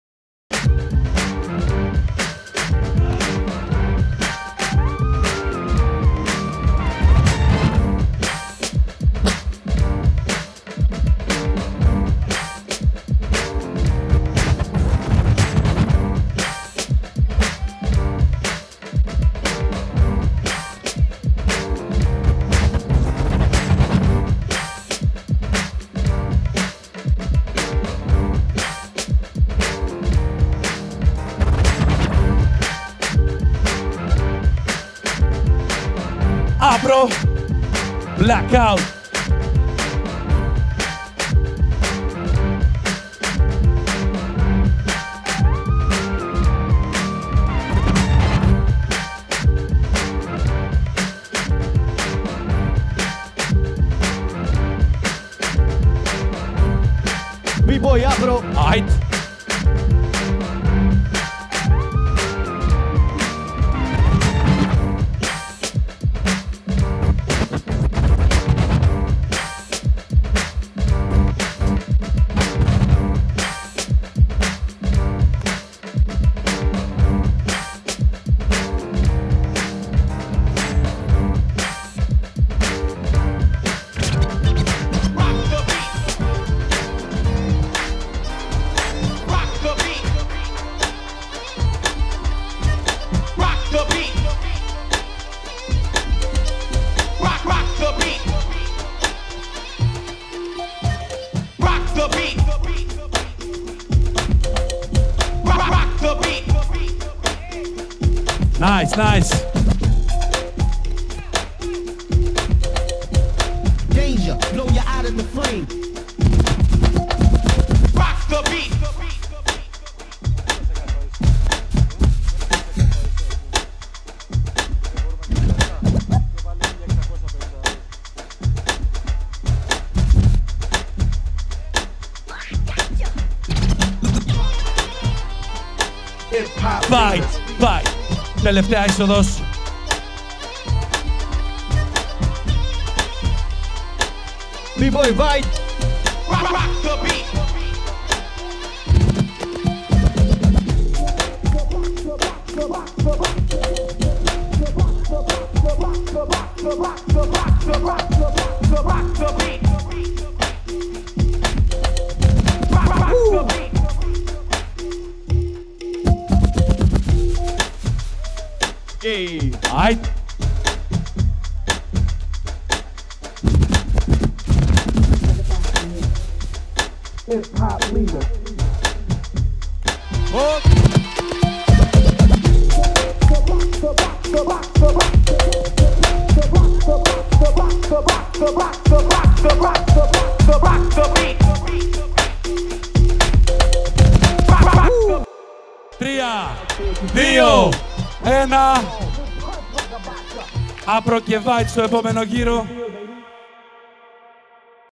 [SouthBreak Jam] 2vs2 Breaking Top16 _ Hwarang & Denny vs Apro & Vide
SouthBreak Jam 2021
2vs2 Top16
Video and Lighting Director : Manolis Levedelis
Camera B : Kostas Kosmadakis